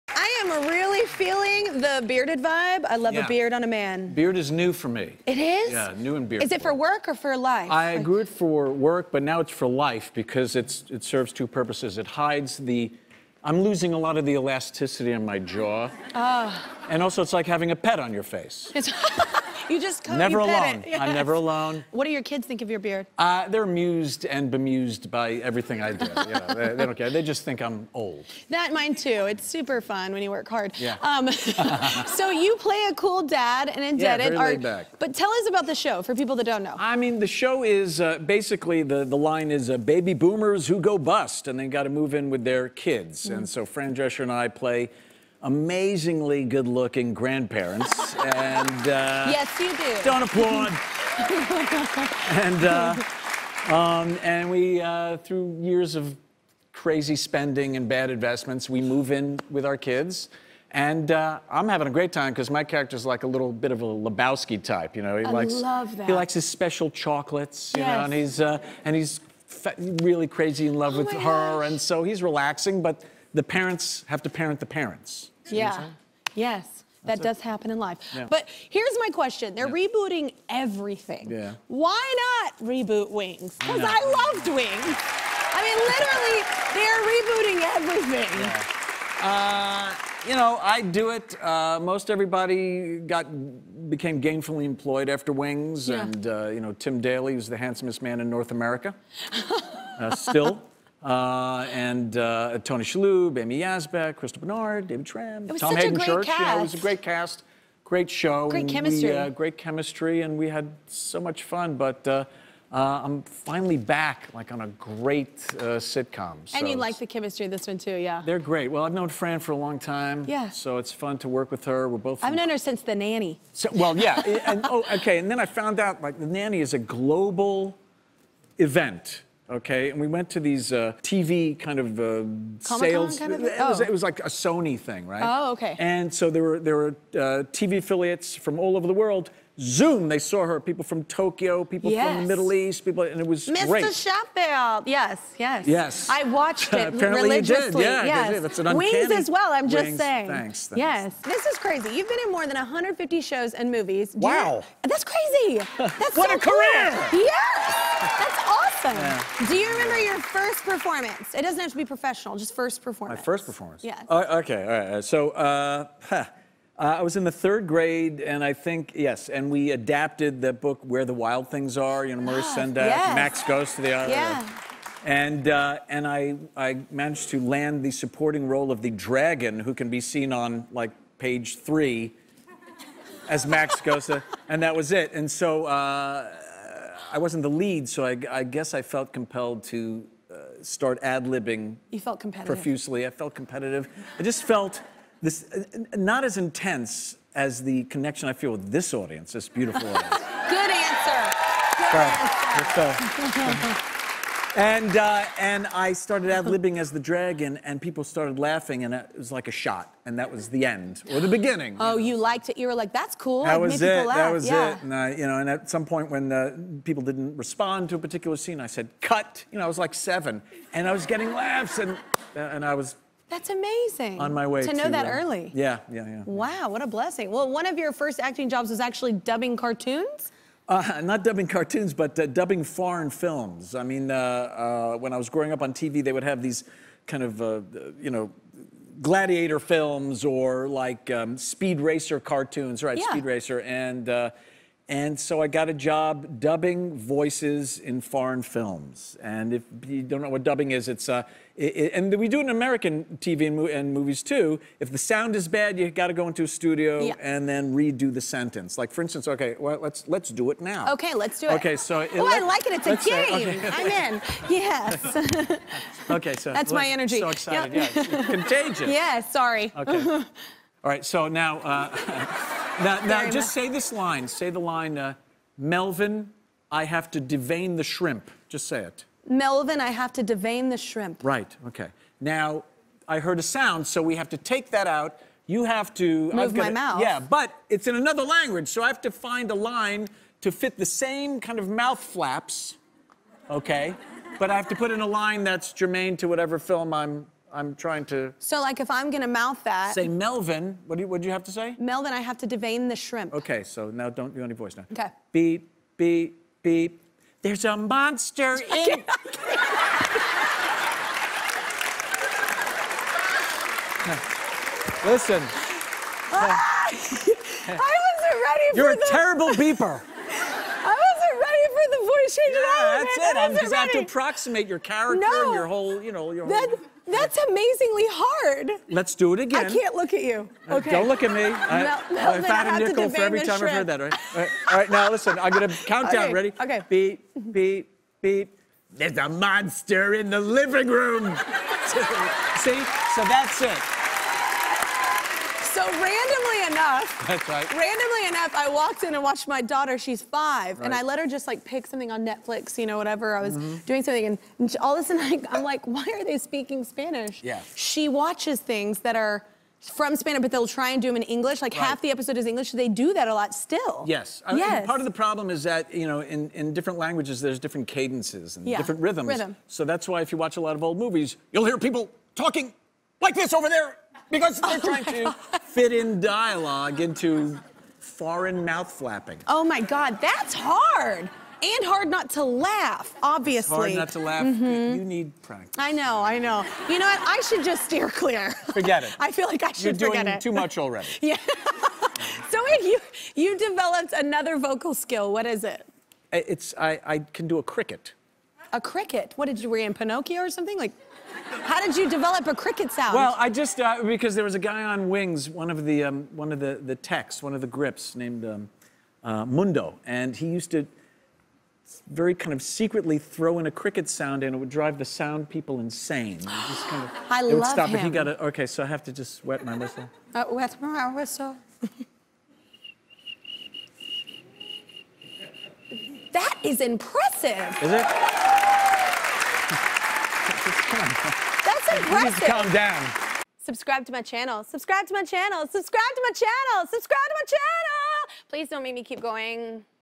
Steven Weber Pranked 'Wings' Crew With Fake Cricket Sound
Steven Weber shows off his unusual talent of imitating the sound of a cricket and reveals that he once used the skill to prank the cast and crew of his sitcom "Wings."  Kelly Clarkson is totally impressed by his special skill.